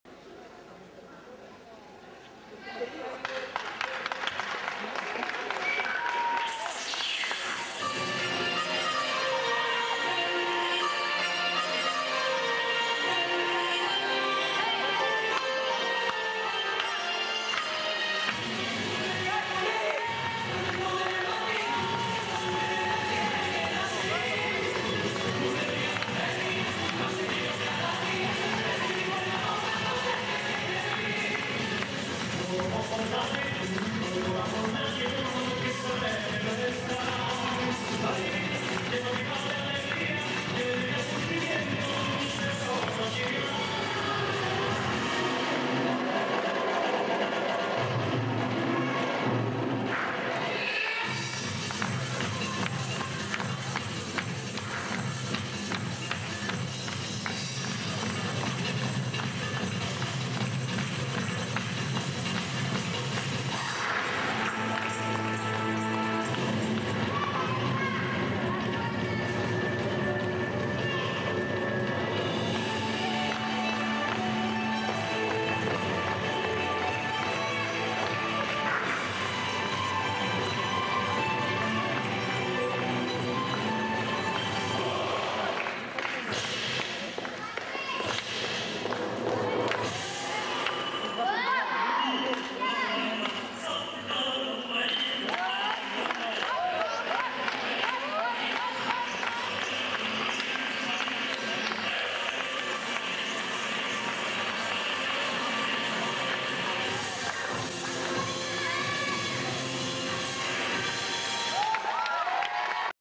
GR - Russia-1 - Sofia 2005 - 1 place
FIG World Series Competition - Sofia 2005 - Russian Group 1 - Amosova, Anisimova, Grishin, Kurochkina, Golenko, Cherepanova - 1 place